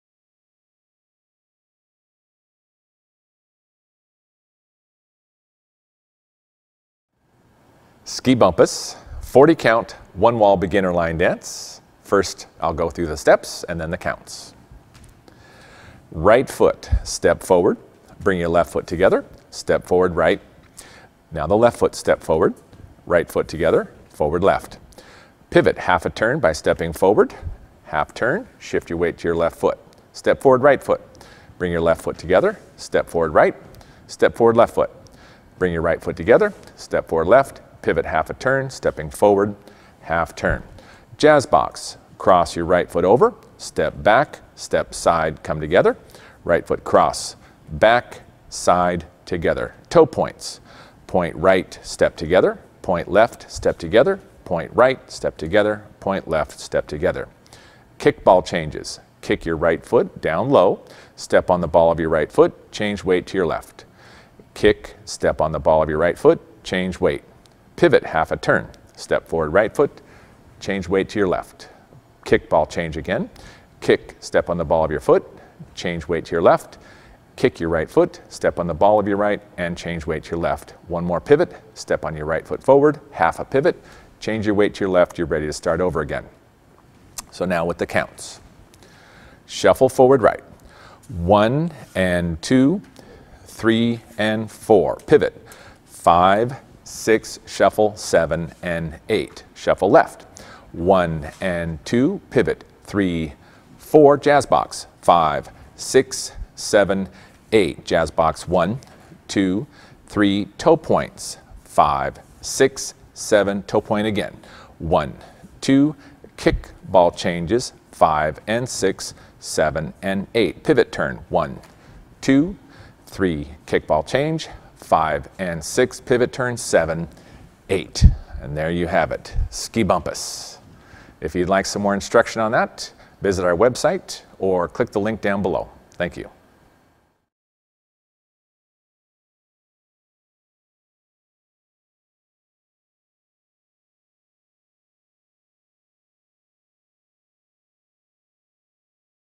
Ski Bumpus - Line Dance Lesson
40 count - 1 wall - Beginner Level Line Dance
Choreographer: Linda De Ford
Instructor: Dave Serfling - Rockin' Horse Dance Barn